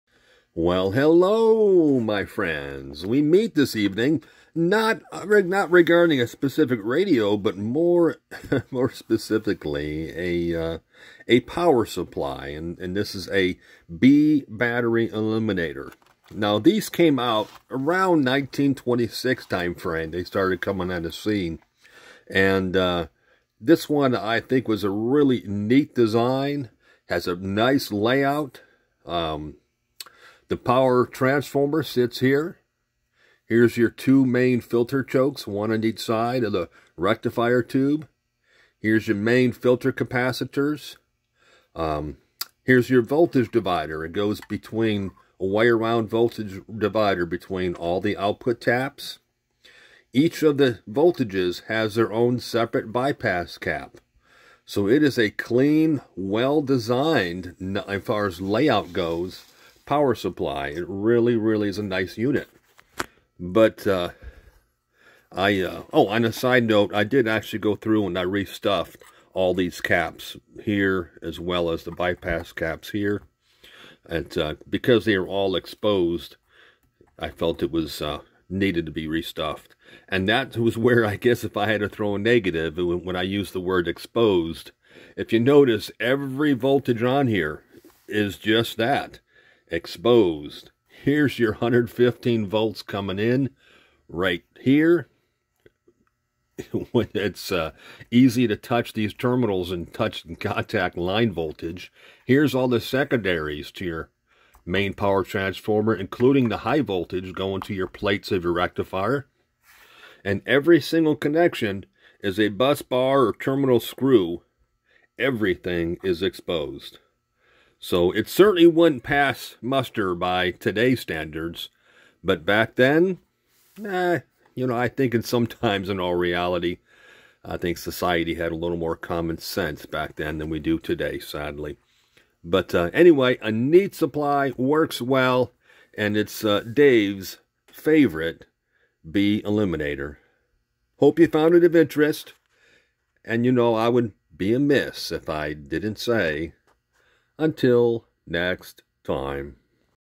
B battery eliminator
These eliminators replicated the batteries required to operate 1920s radios.